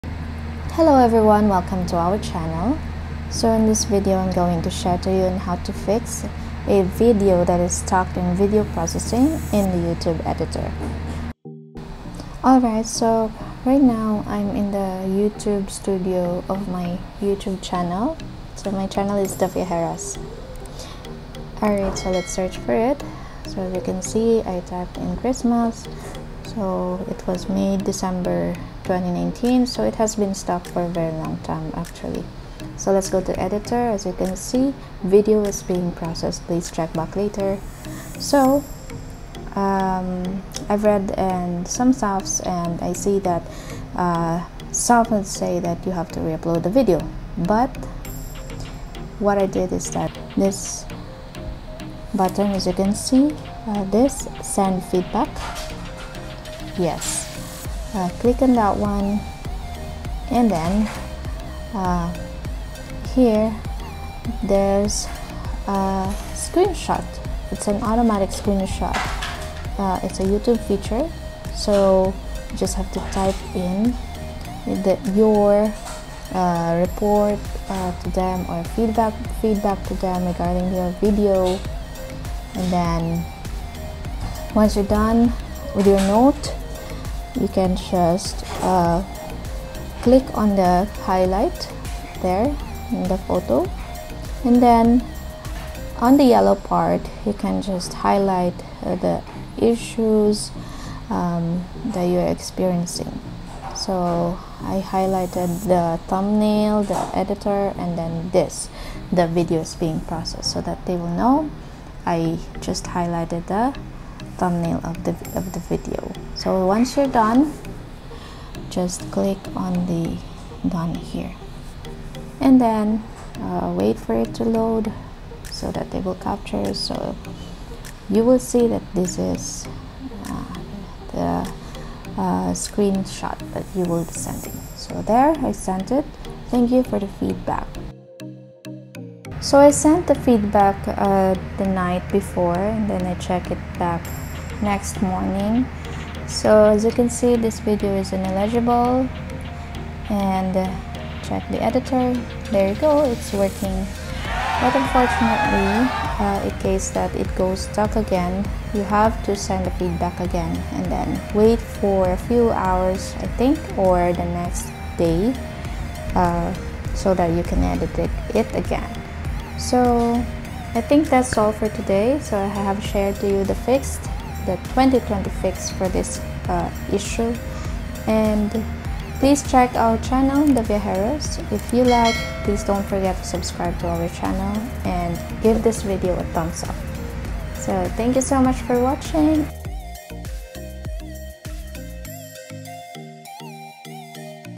HOW TO FIX A VIDEO STUCK IN EDITOR PROCESSING FOR A LONG TIME (2020) NEW YOUTUBE VIDEO EDITOR
NOTE: This fix is only for videos that are stuck in processing in the Youtube EDITOR NOT for videos stuck when uploading. Please find the right video for that.

To my co-youtube creators, I just want to share this workaround on how you can fix a video that is stuck in video processing for a long time in the new Youtube Editor.